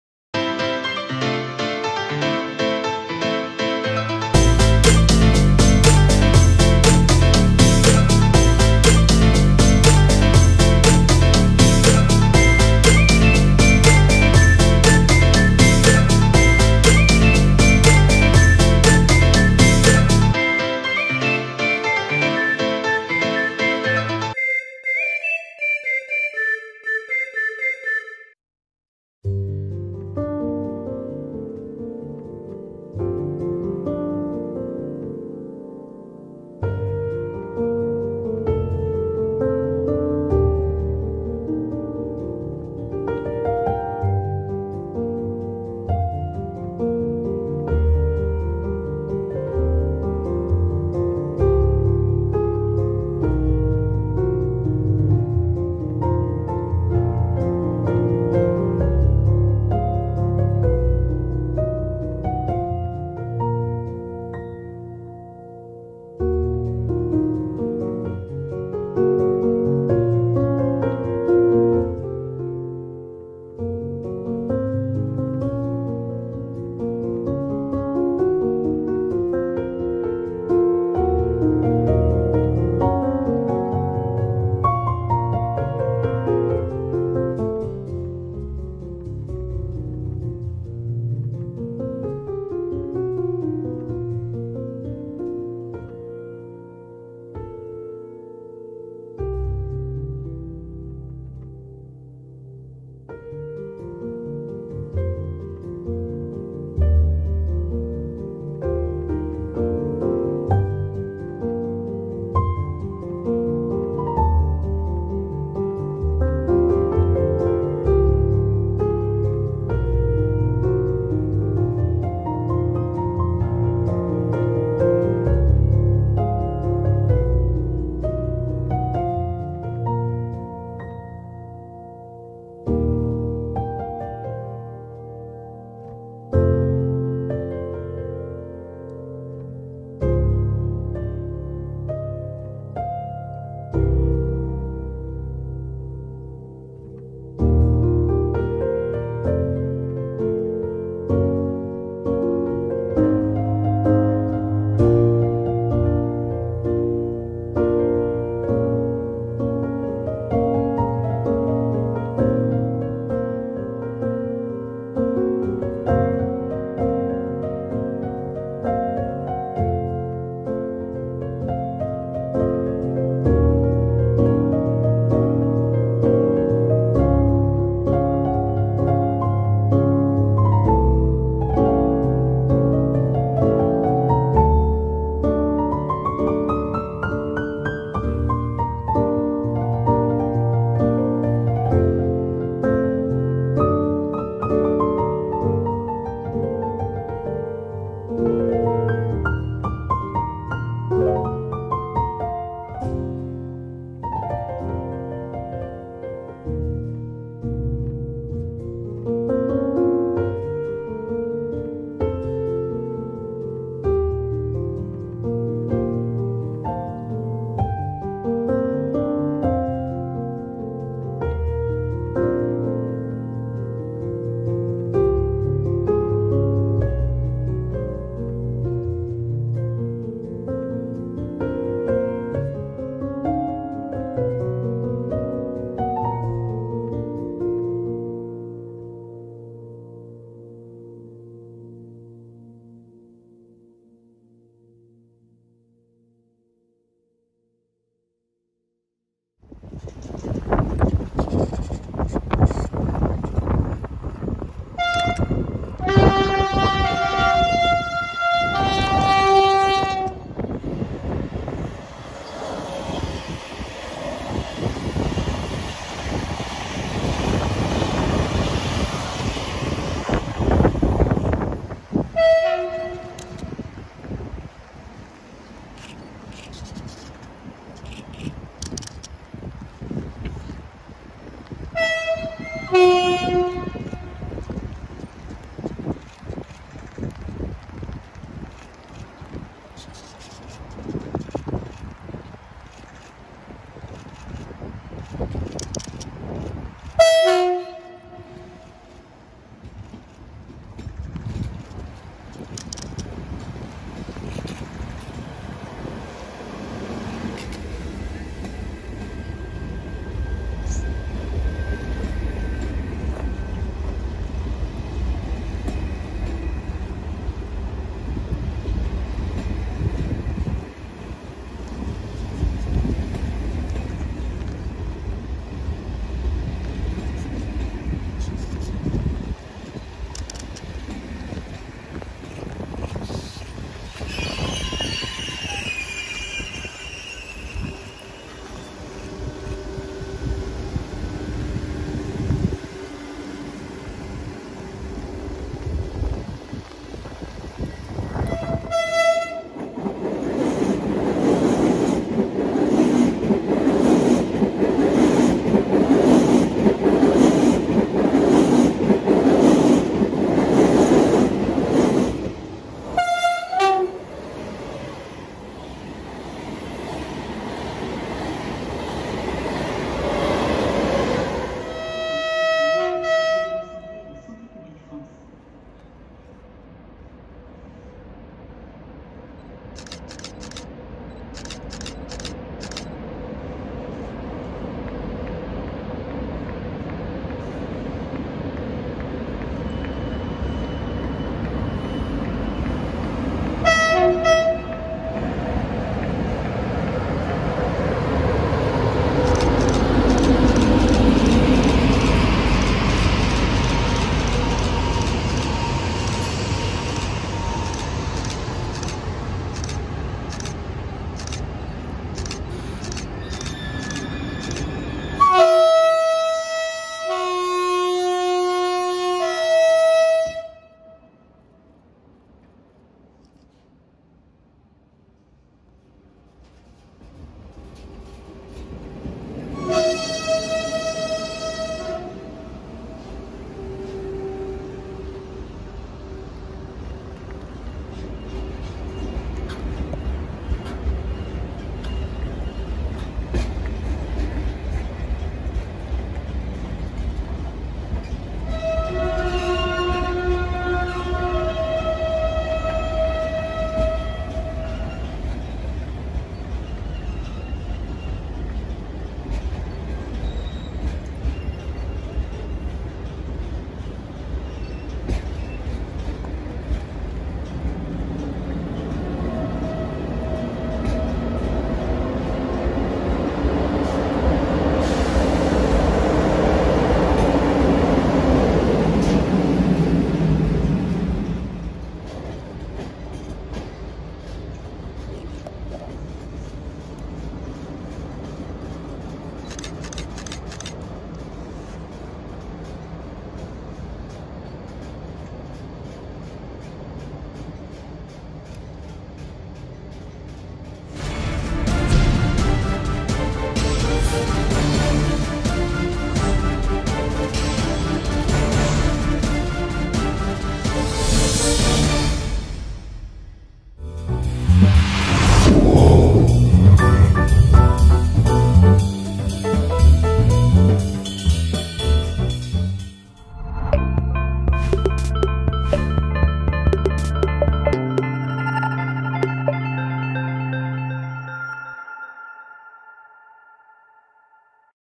Au Revoir Patrick, le TGV PSE 01 #488
Une vidéo qui retrace l’histoire des débuts du TGV, et du dernier TGV Paris Sud-Est « Patrick »

Bon visionnage